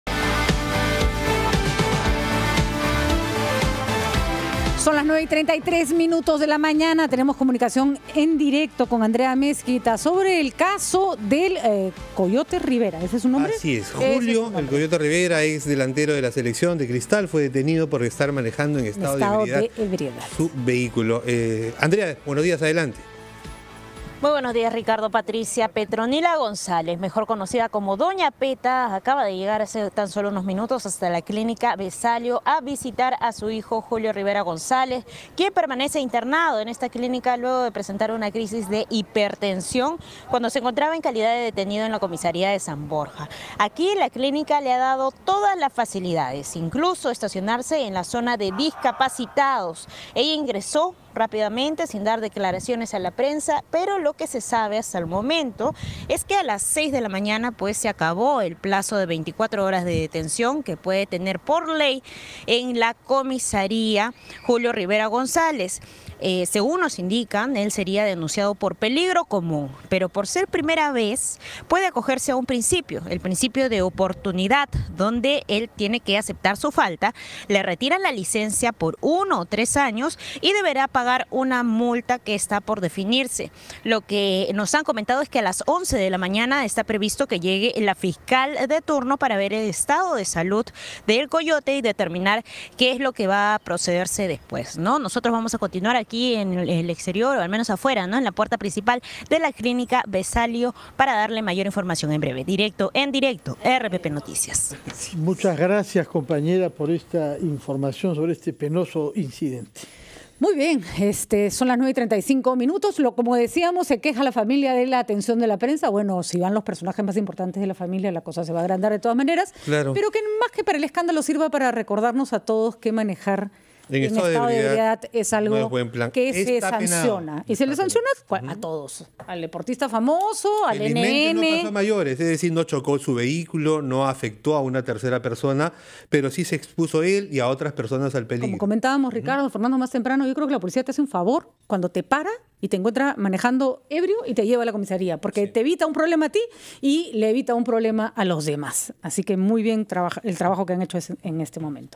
Doña 'Peta' llegó a clínica para visitar a su hijo Julio 'Coyote' Rivera
Petronila Gonzales, conocida como doña 'Peta', llegó a una clínica local para visitar a su hijo Julio 'Coyote' Rivera quien está internado en el centro médico tras sufrir una crisis de hipertensión cuando permanecía detenido en la comisaría de San Borja por conducir en estado de ebriedad